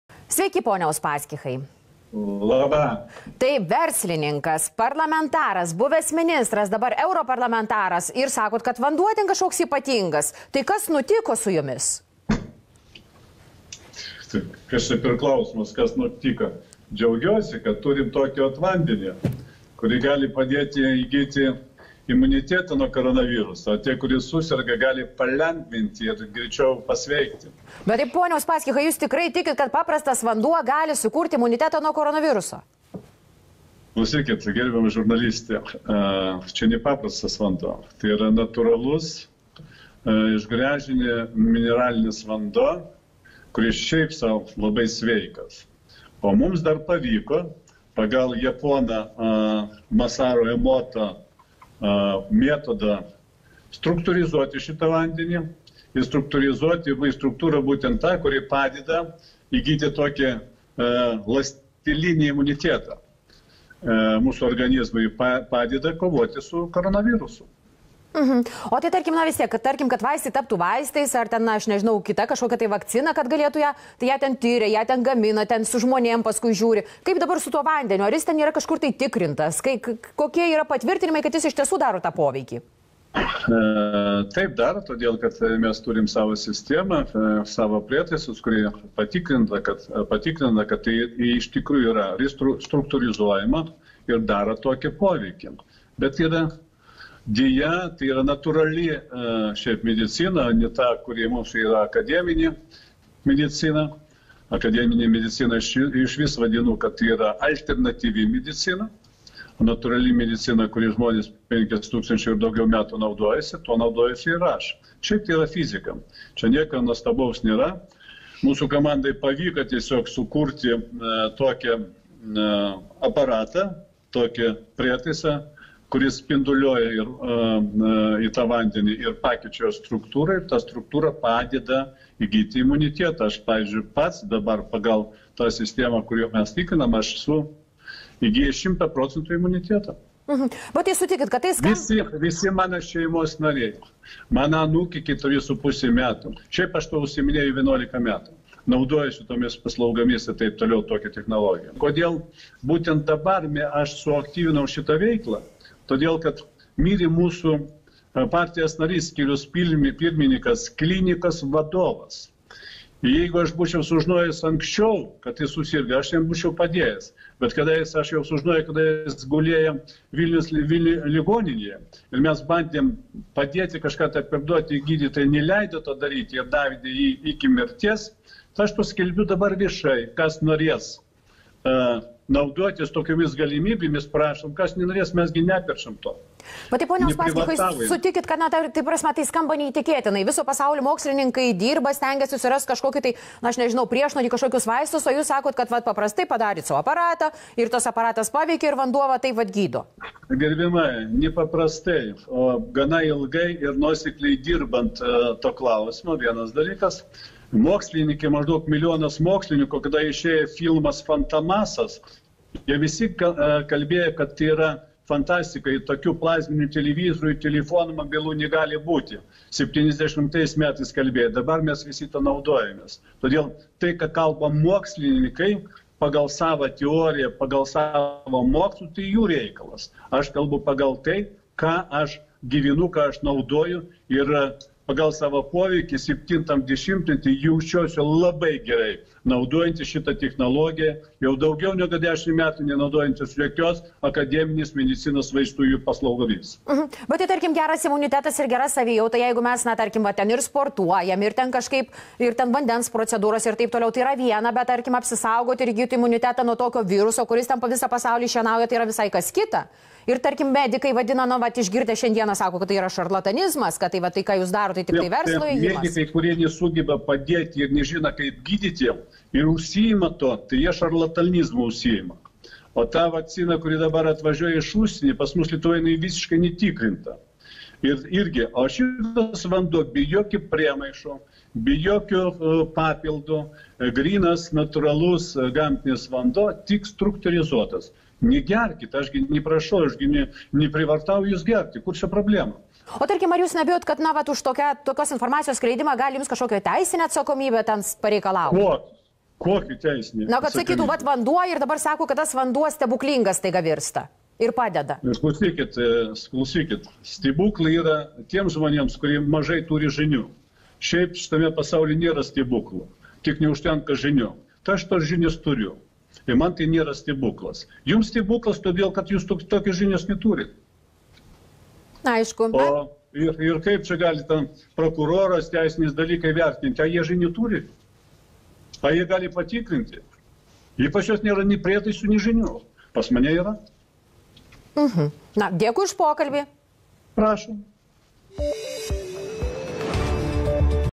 Viktor Uspaskich EU-MP Claims to Have Invented COVID-19 Curing Water (English Subtitles)
A Lithuanian/Russian multi-millionaire, member of European Parliament and a Business man, strongly claims that he has invented all curing water on national television, whilst calling doctors and scientists charlatans who cannot help anyone and that normal medicine is actually alternative medicine. Unfortunately, this is not April fools.